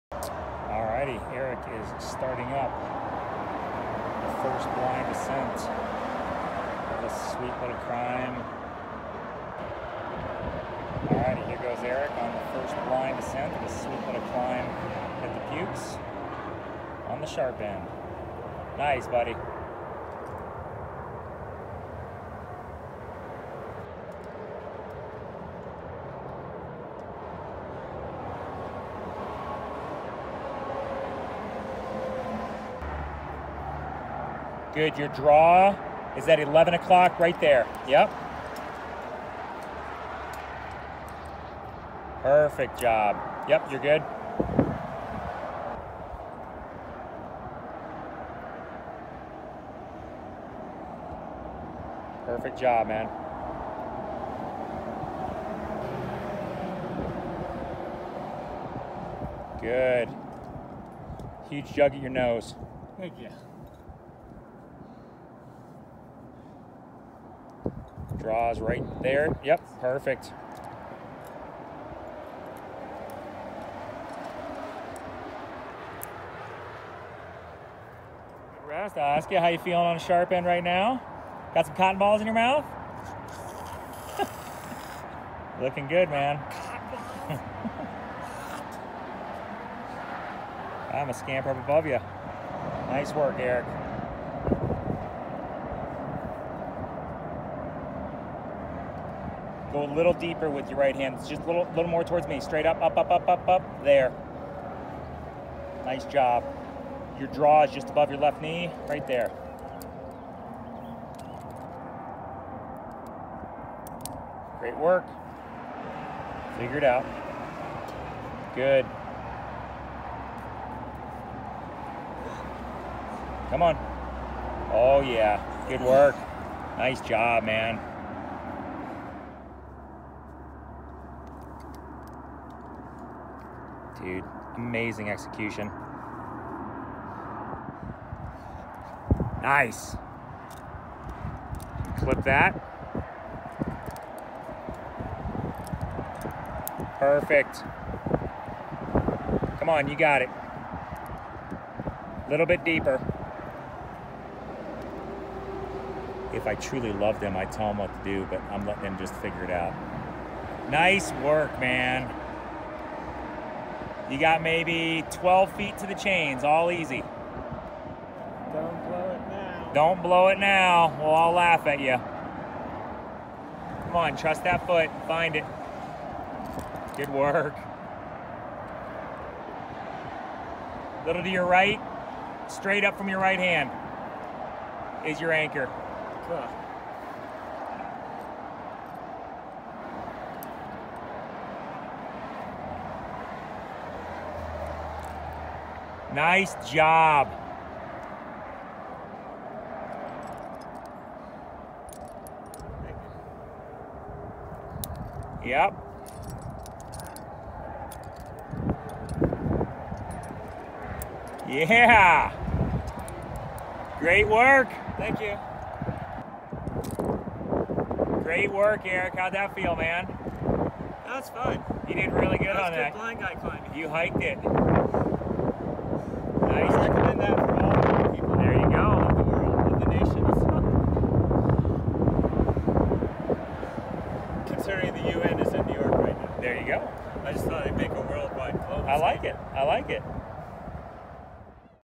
Erik Weihenmayer Climbs the Puoux
Erik Weihenmayer, one of the most inspirational people on the planet, climbs a limestone route in The Puoux. Glenwood Canyon, Colorado. Filmed by Mike Brumbaugh.